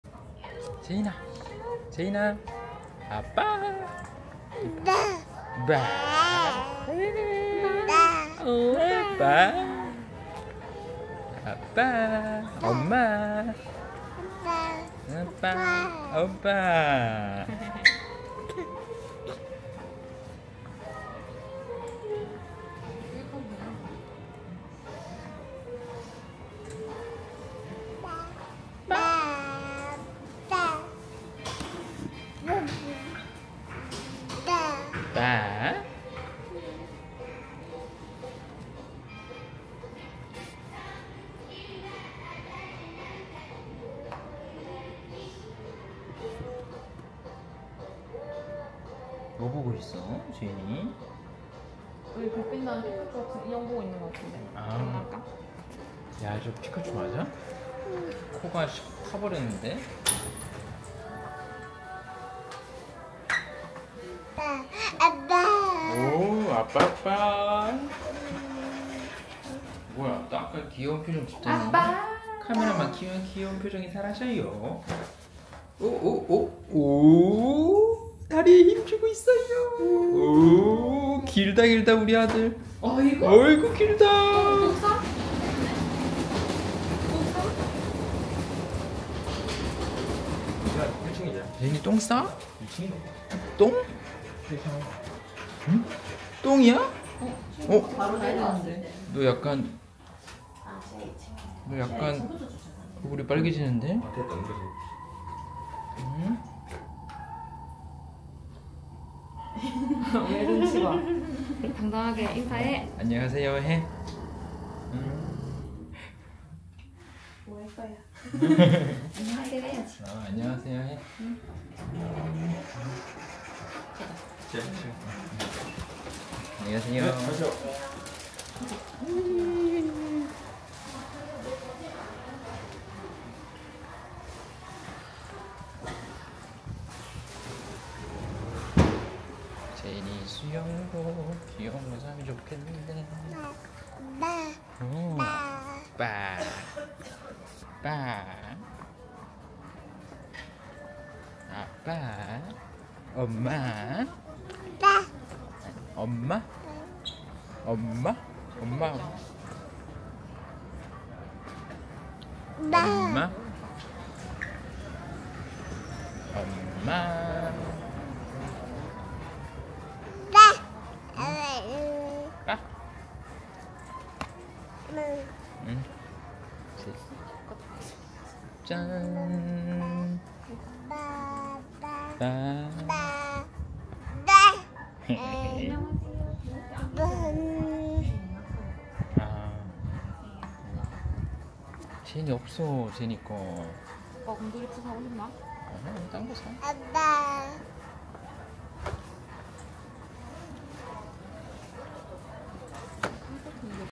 2024.08.20. 재인이 294일차 수영복 사러 아울렛 나들이
바로 코앞이라 아주 자주 감ㅋ